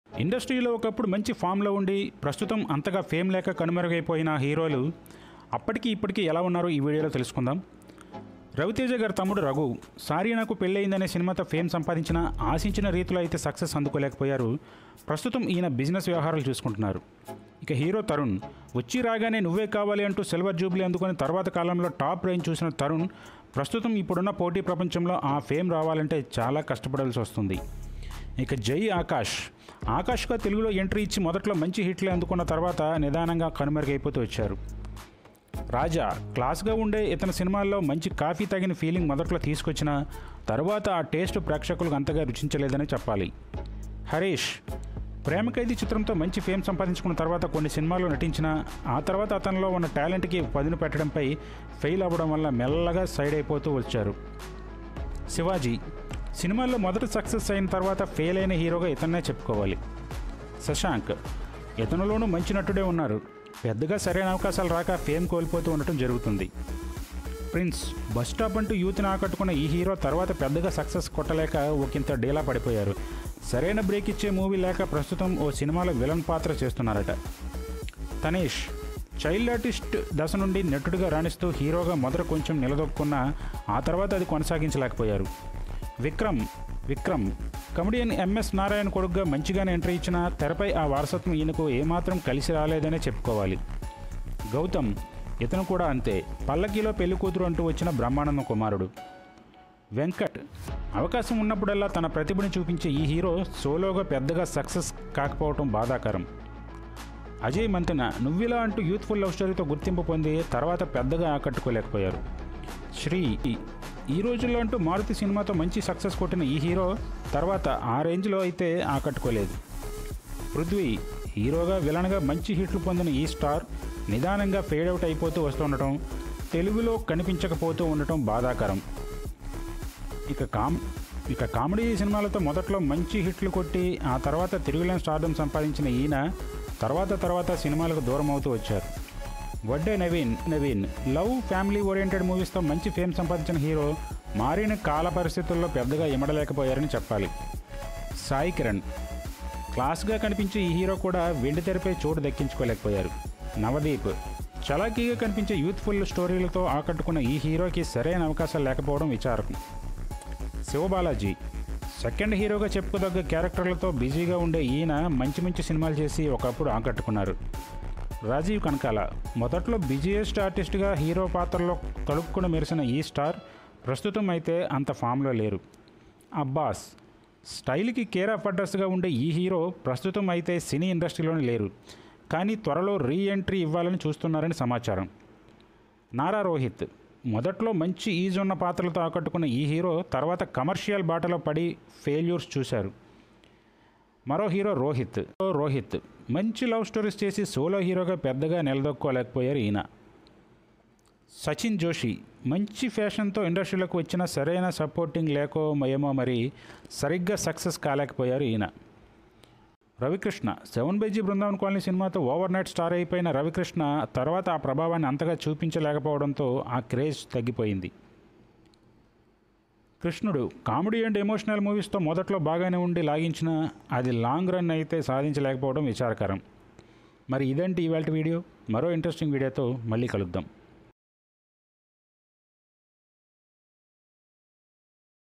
Tollywood Heroes Then and Now | Yesteryear Actors | Fade out Actors | Tollywood Stuff
actor prudhvi, who acted in pelli movie telugu and preyasi rave film telugu. actor raja, who worked in telugu , anand o manchi coffee lanti cinema. actor, tanish acted in telugu movies, sivaji telugu movies are missamma, tajmahal , vadde navven, who acted in pelli movie, korukunna priyudu, snehithudu, satruvu movie, oka oorilo movie. telugu movies from actor harish kumar are : prema khaidi, daddy daddy, college kurradu, pellaindi kani. and mass maharaj ravi teja brother raghu's telugu film was sorry naku pellaindi. hero venu telugu films, pellam oorelithe along with co actor srikanth and comedian suneel. and venu blockbuster telugu movie hanuman junction, along with action king arjun and jagapathi babu.

Tollywood Stuff